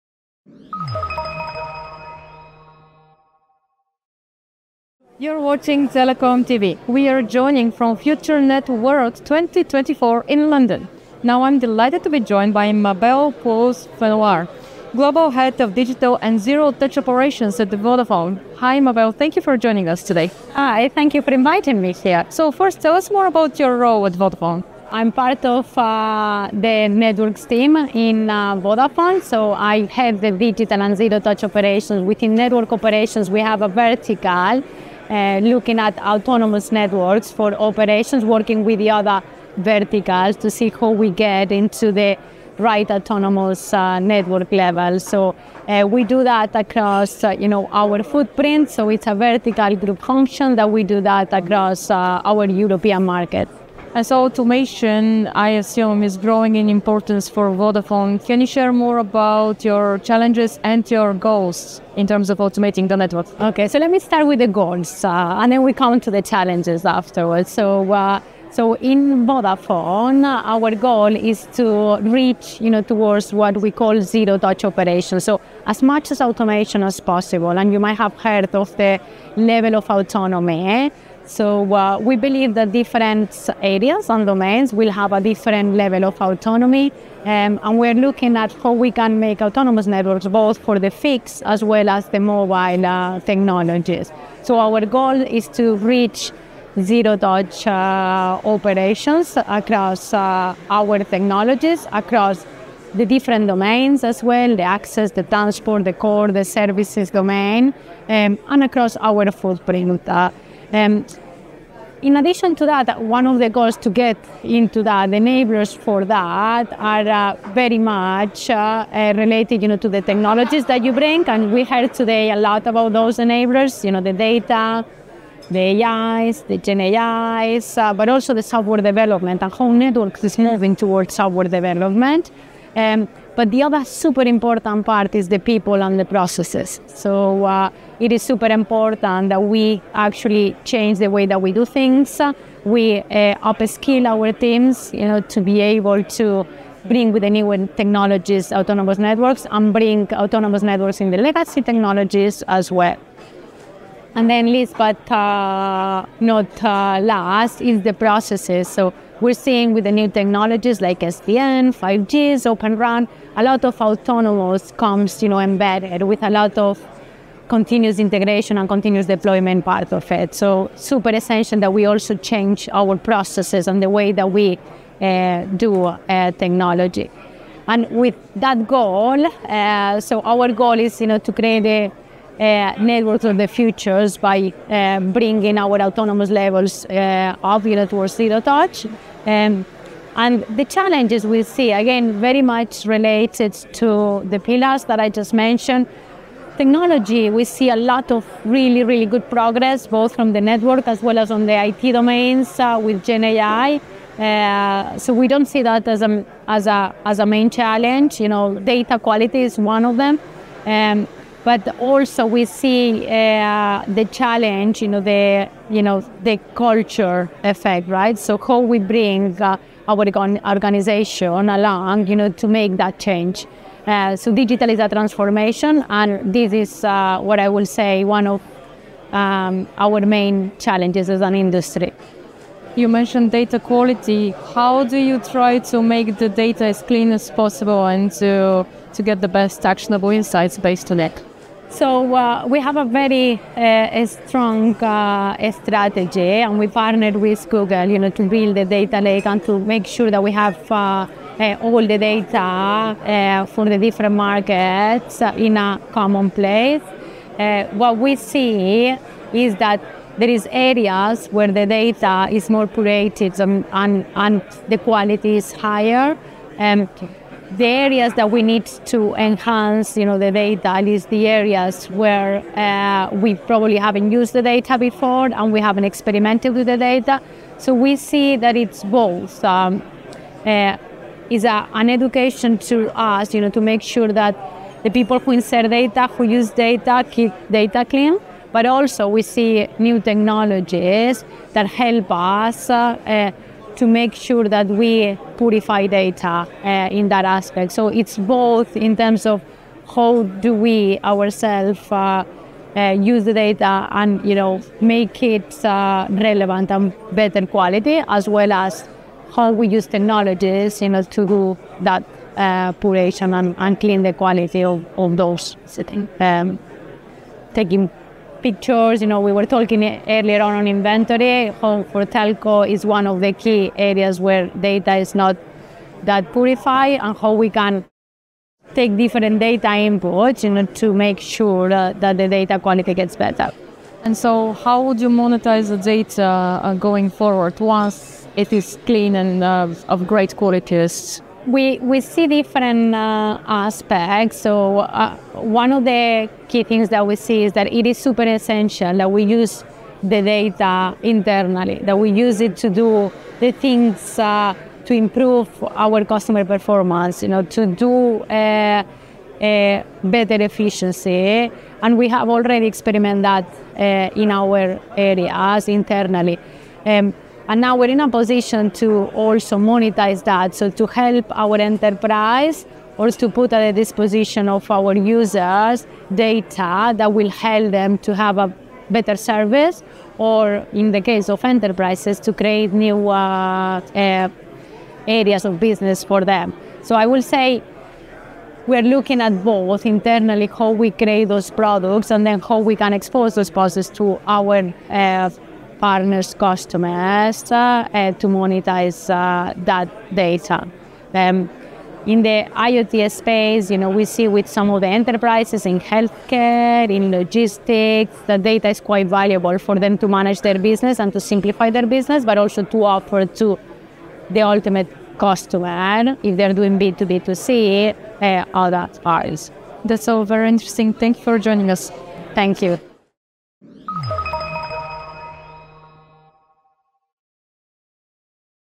Vodafone’s Mabel Pous-Fenollar on network automation
During the FutureNet World 2024 event in London, TelecomTV caught up with Mabel Pous-Fenollar, global head of digital and zero-touch operations at Vodafone, to learn about the operator’s network automation strategy. She noted the importance of people and processes, as well as technology innovation, in achieving greater automation and pointed out that clean, high-quality data is vital to unlocking new business opportunities in the enterprise services sector.